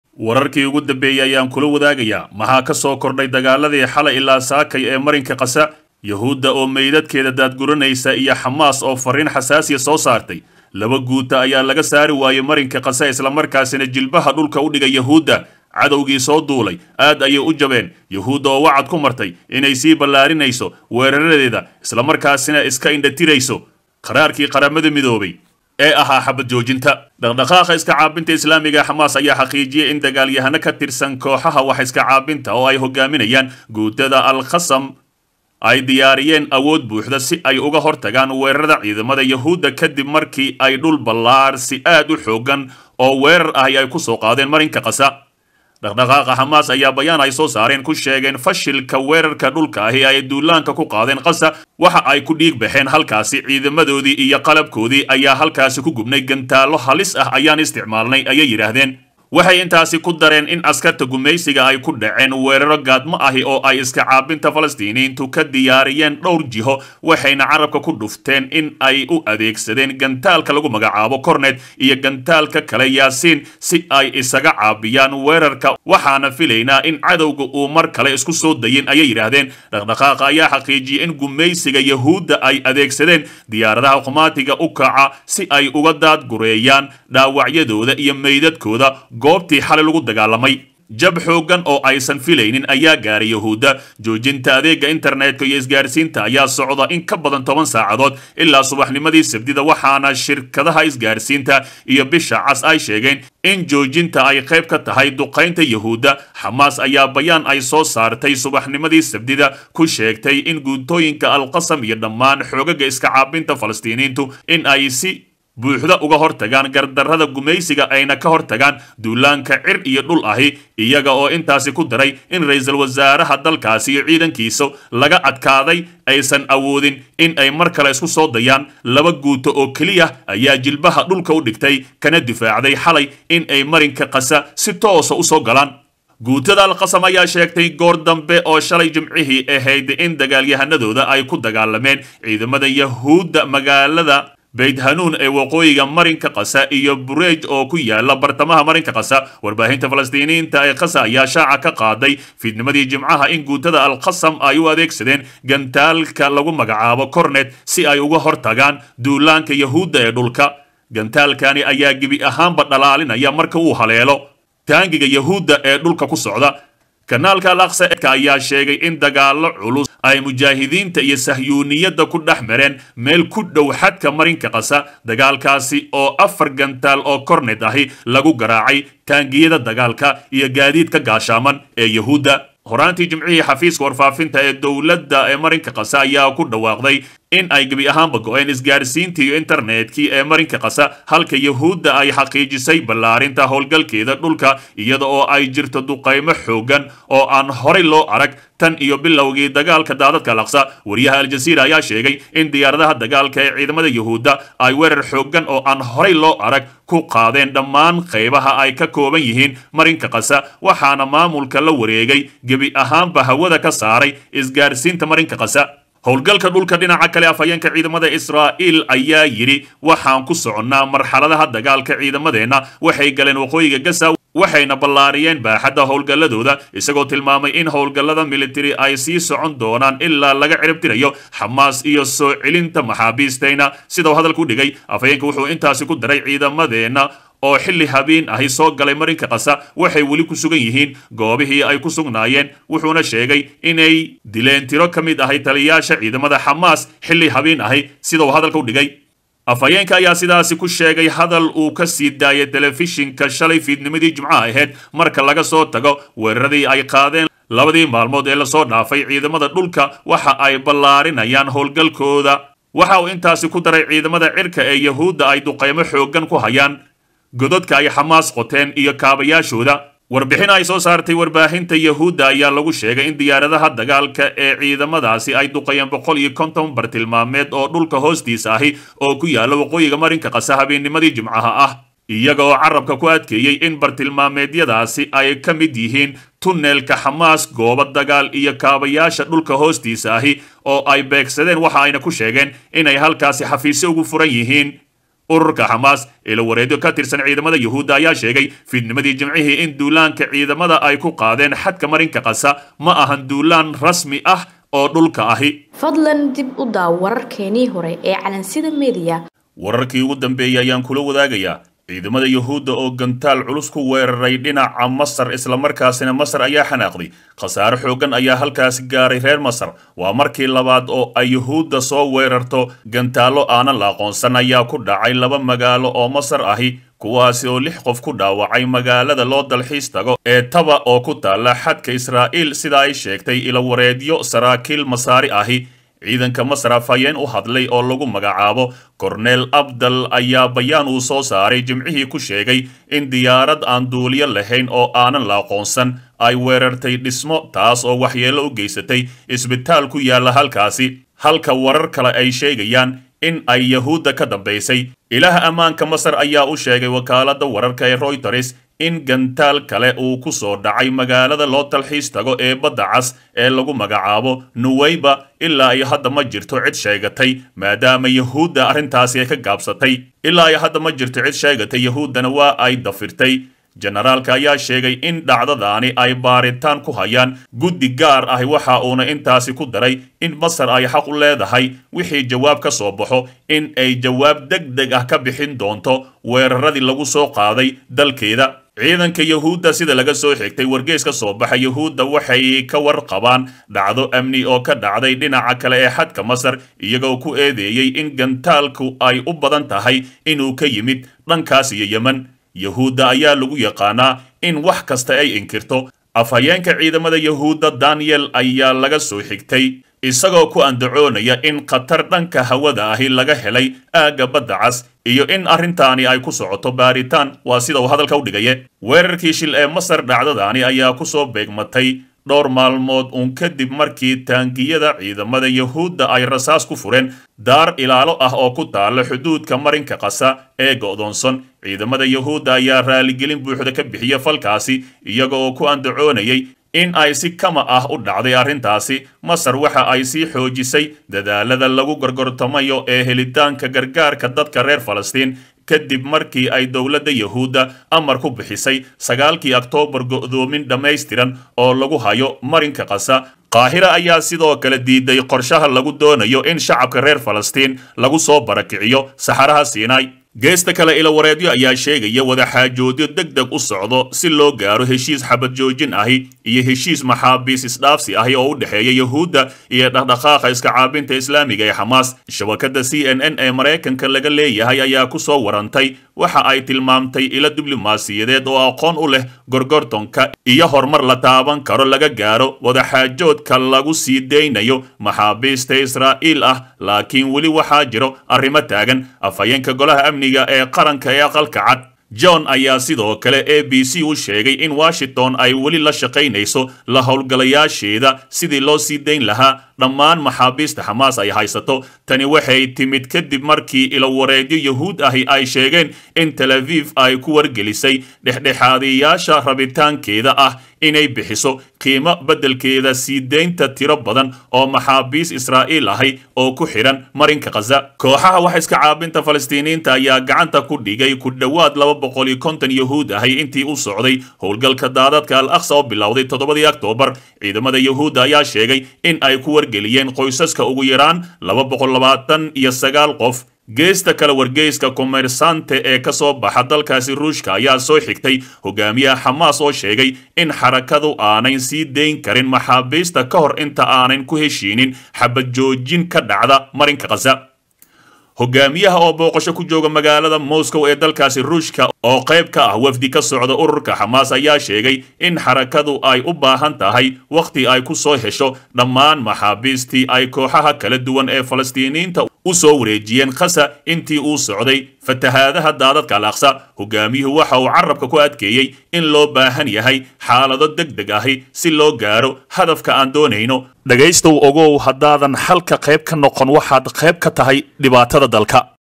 DEG DEG Dhaawacyada Yahuuda oo diyaarado lagu Daad gureynayo & Guusha xamas 2 guuto oo laga bixin wa
DEG DEG Wararkii ugu dambeyay Dhaawacyada Yahuuda oo Diyaarado lagu daad Gureynayo Halaag culus loo geystay Guusha Xamaas labo Guuto oo laga bixin waayay Waqooyiga GAZA ( Duceey walaalahaa Falastin)